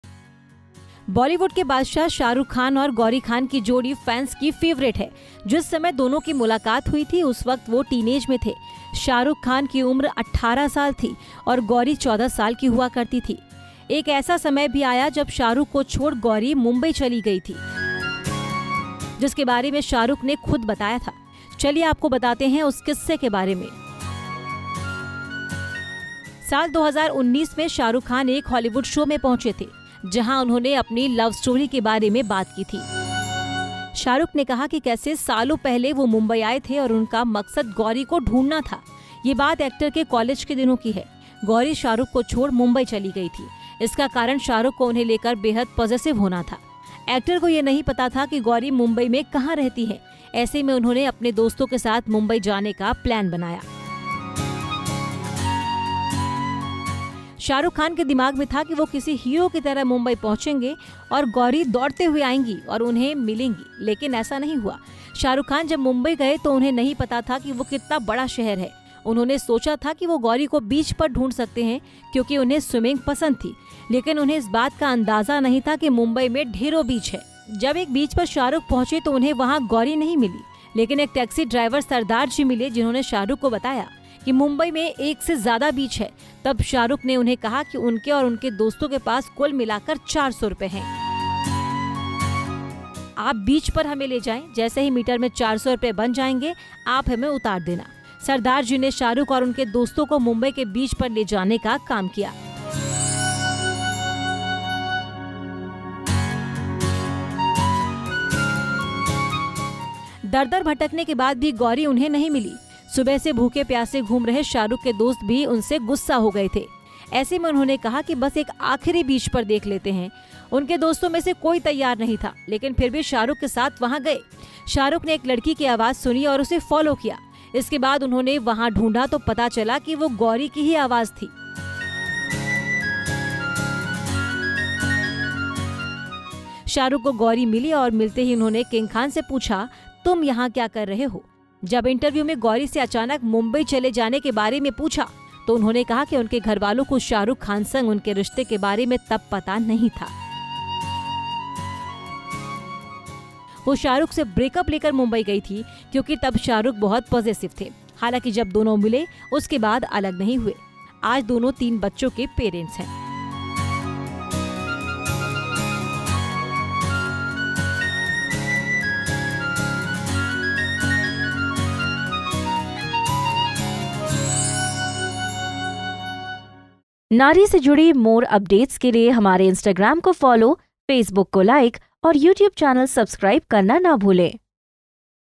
ना घर का पता, ना ही फोन नंबर, बीवी Gauri Khan को ढूंढने निकले थे Shahrukh Khan
ना घर का पता, ना ही फोन नंबर, बीवी Gauri Khan को ढूंढने निकले थे Shahrukh Khan, भूखे-प्यासे समंदर किनारे SRK को देख गौरी रह गई थी हैरान.

Neither home address, nor phone number, Shahrukh Khan had gone out to find wife Gauri Khan, Gauri was shocked to see SRK on the seashore hungry and thirsty.